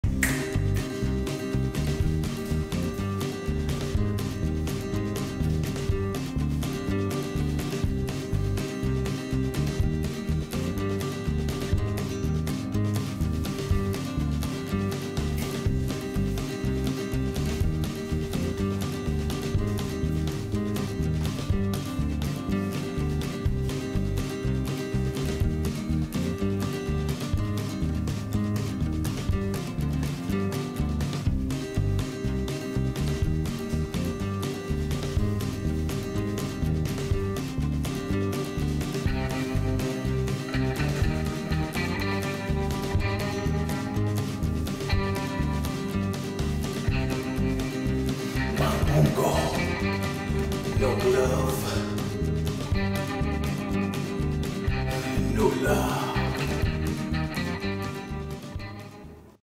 Manbungo visits Massive Heart Attack victim,Discosean21
How nice of the superstar to go visit discosean21 in his time of need.He dancing and shit,just have a  good old time.He in there act like a 200% nigga  music video,lol.